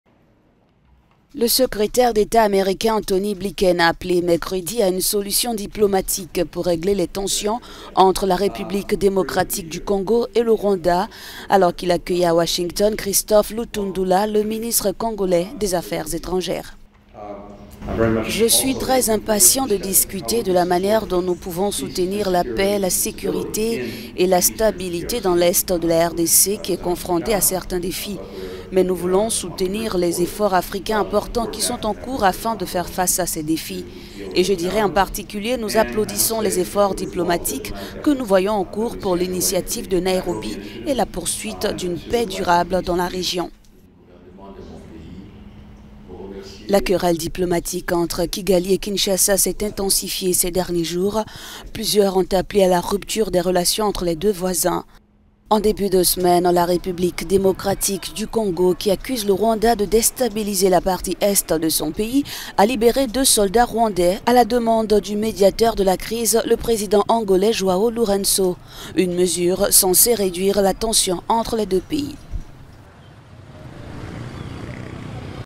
RDC-Rwanda : Washington appelle à la diplomatie pour apaiser les tensions
Il a notamment salué les discussions qui se sont tenues en avril entre le gouvernement congolais et des groupes rebelles à Nairobi, au Kenya.